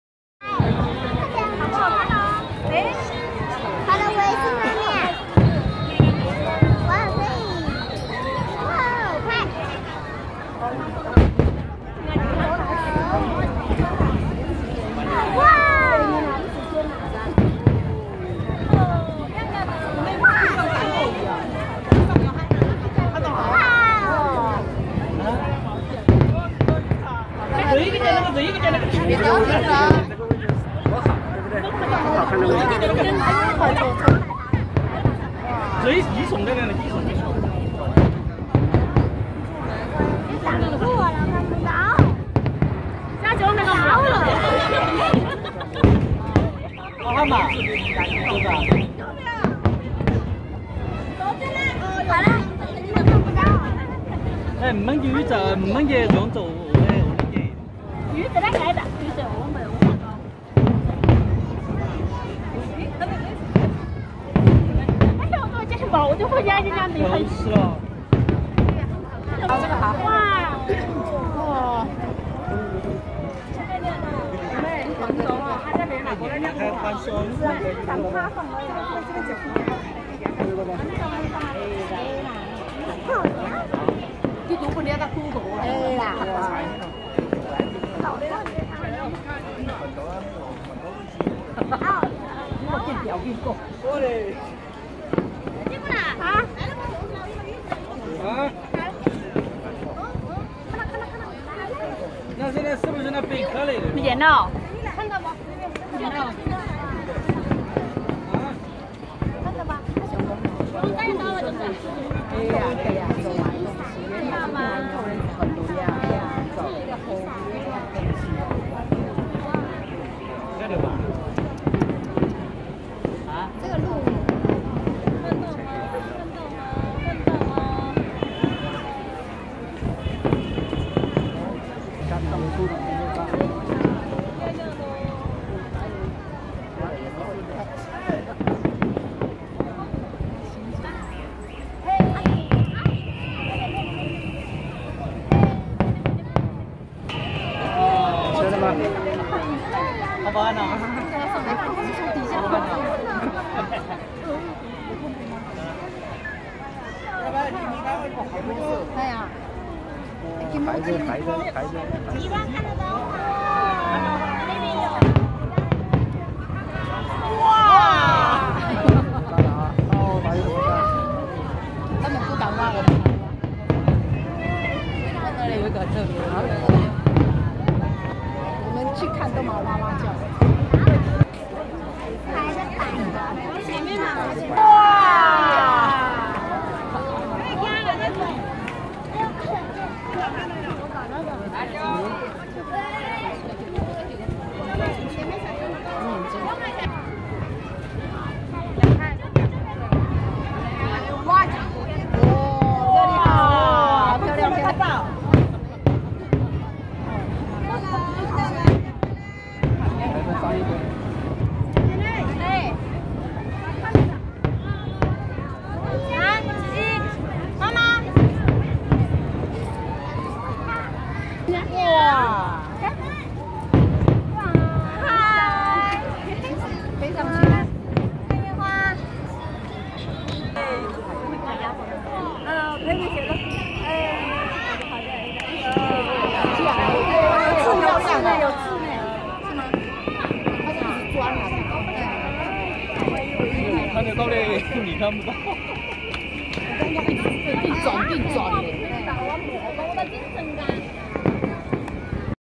7/16日張家界之旅 欣賞火花秀
7/16日張家界之旅 第五天晚上欣賞高空火花秀